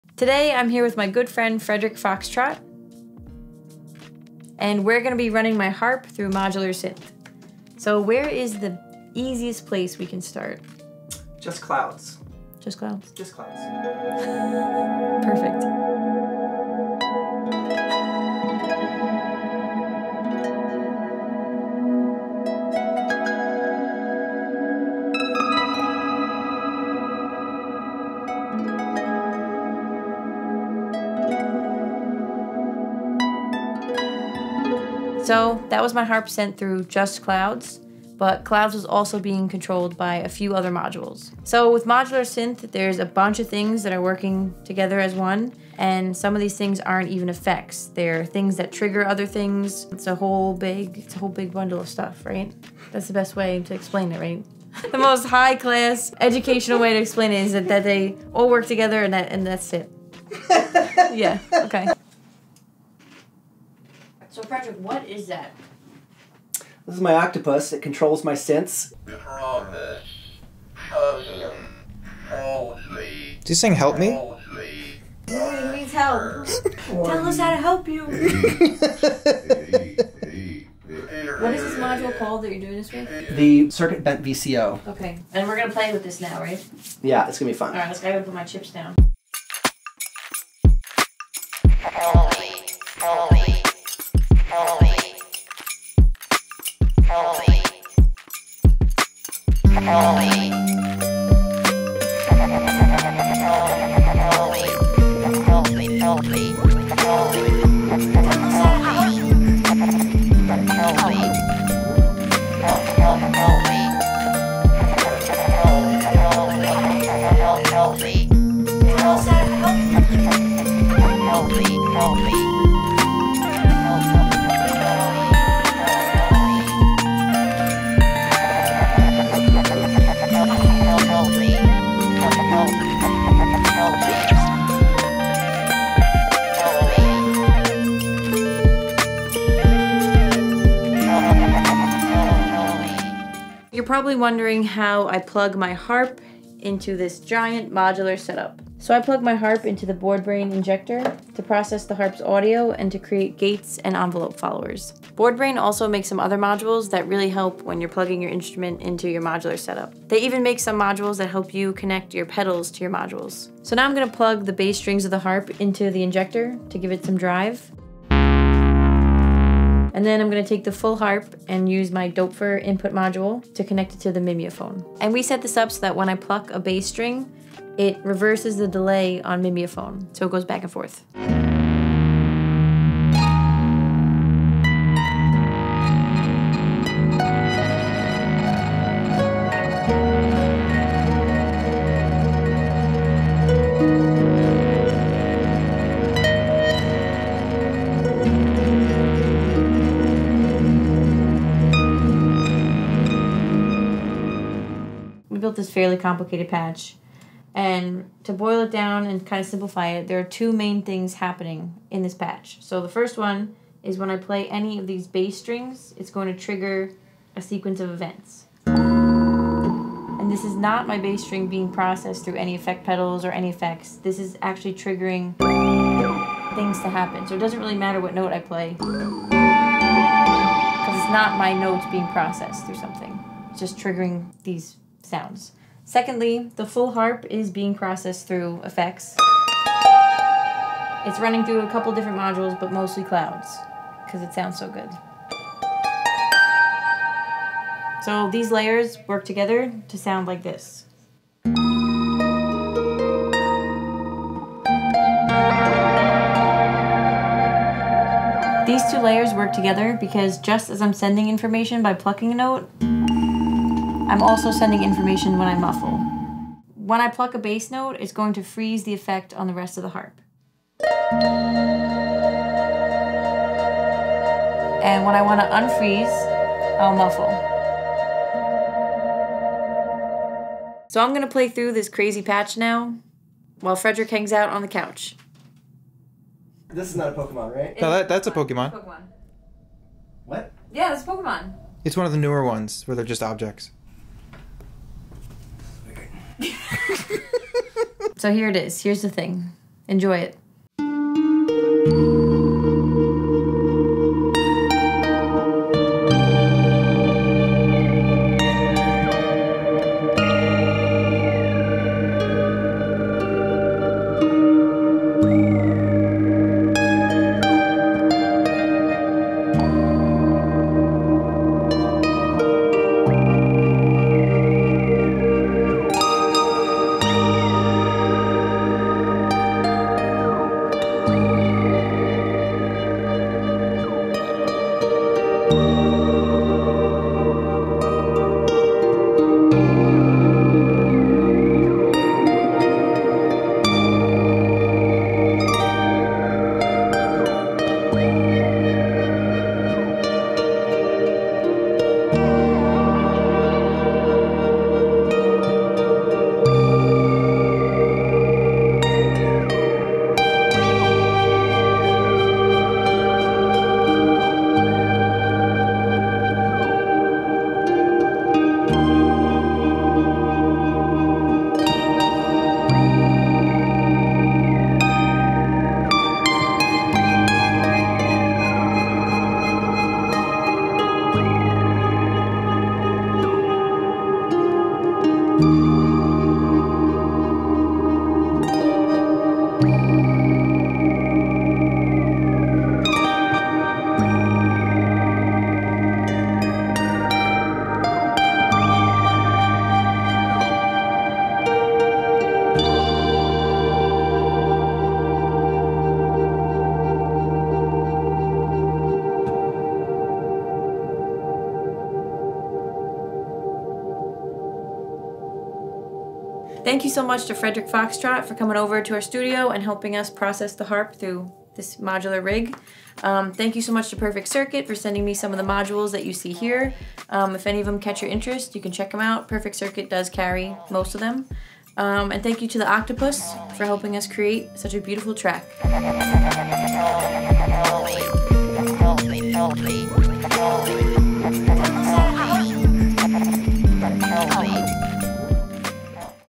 I plugged my harp into modular synth. It was insane.
Harp through modular synth is nuts. Frederick and I had a lot of fun.